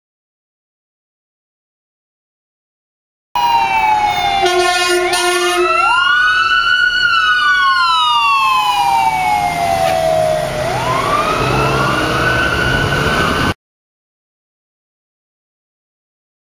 SPFD-RA 81-Code 3
South Pasadena Fire Department rescue ambulance (RA) 81 responding code 3 in Pasadena, CA